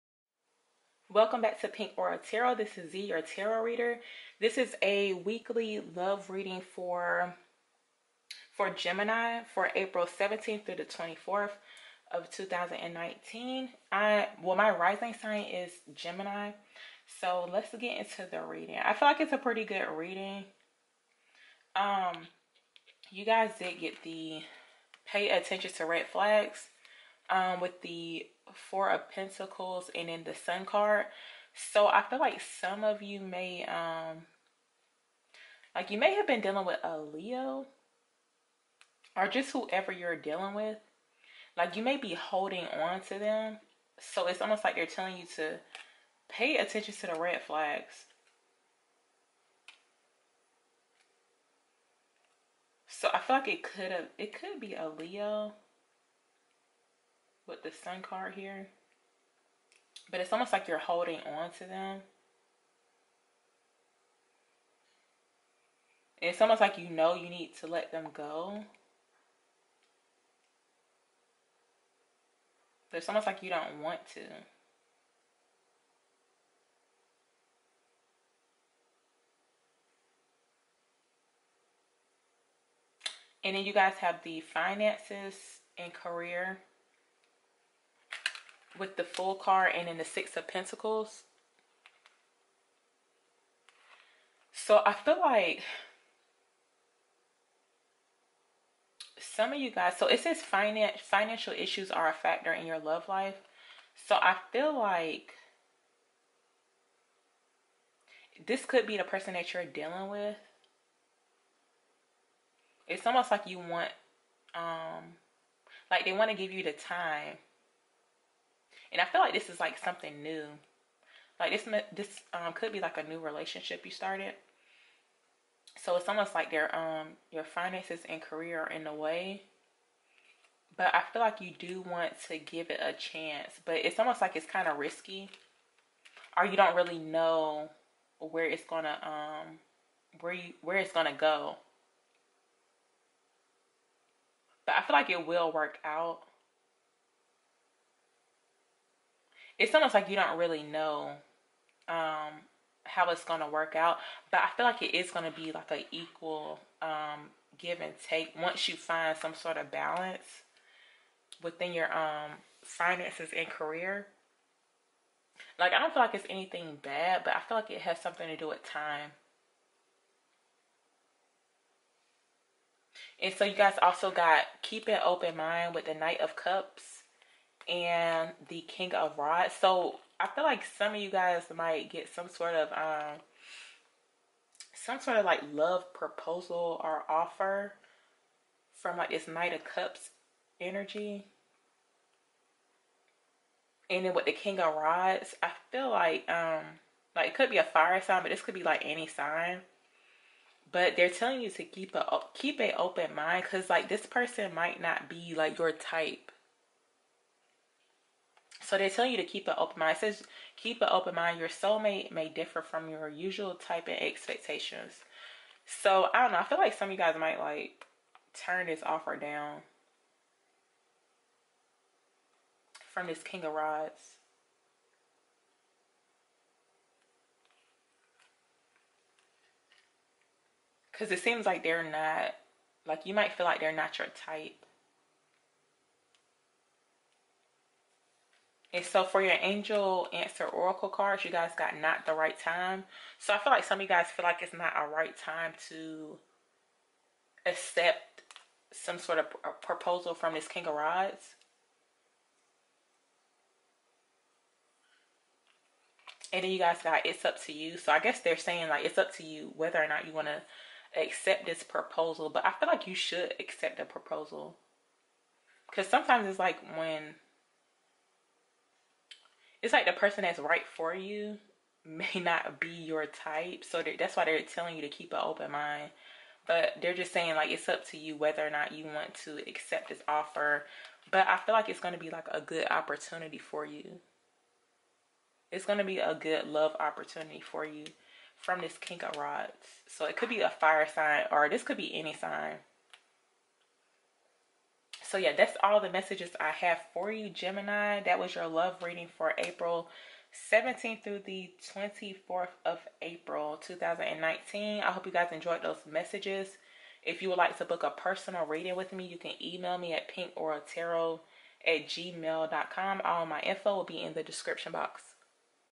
❤️GEMINI ♊️❤️ “Will You Accept This Proposal?” April 17-24 2019 Love Reading
Instagram.com/pinkauratarot

Twitter: Pink Aura Tarot

🌞Personal Readings Available $15🌞